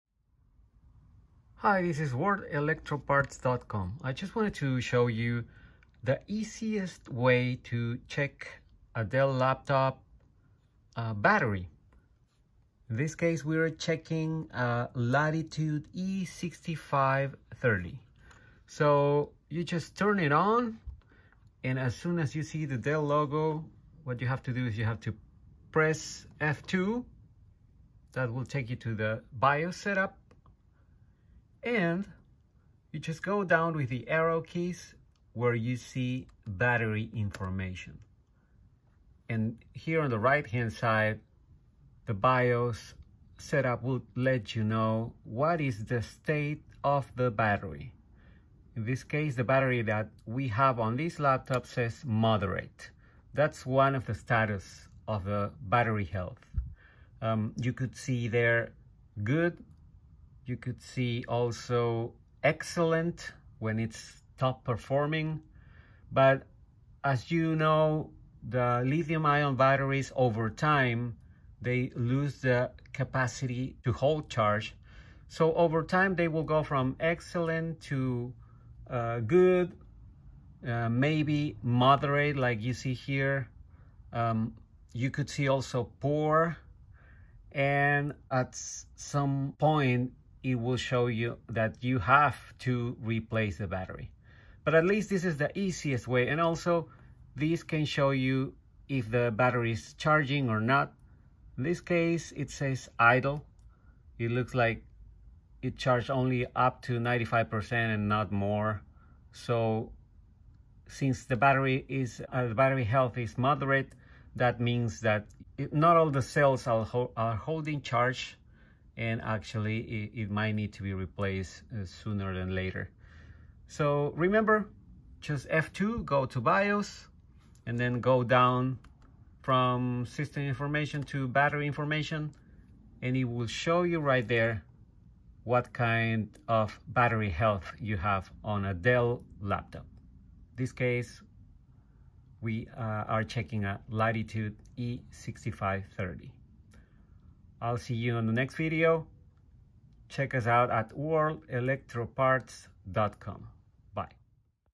Dell Latitude E6530 checking battery - easiest way.
We show you the easiest way to check battery health on a Dell Latitude E6530 laptop computer

Worldelectroparts.com